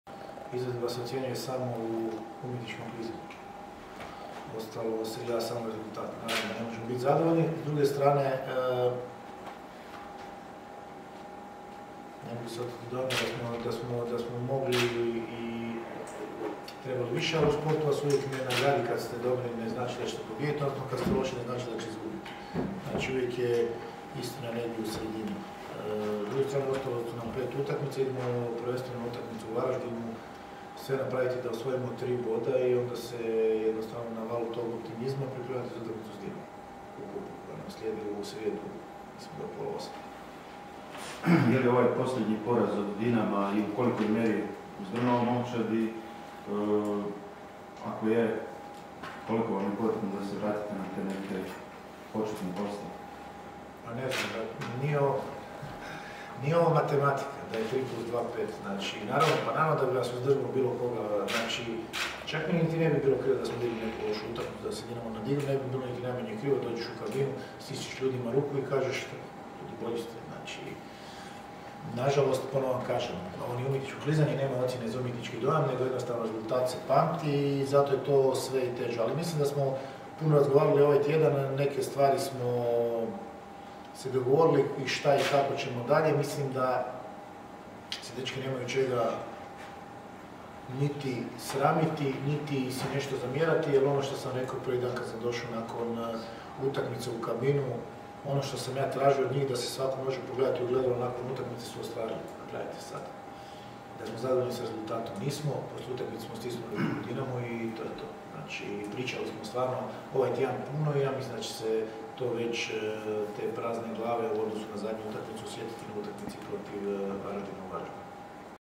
Željko Sopić najavio utakmicu Rijeke i Varaždina
Konferencija za medije uoči nogometne utakmice Varaždin - Rijeka. Utakmicu su najavili trener Željko Sopić i igrač Marijan Čabraja.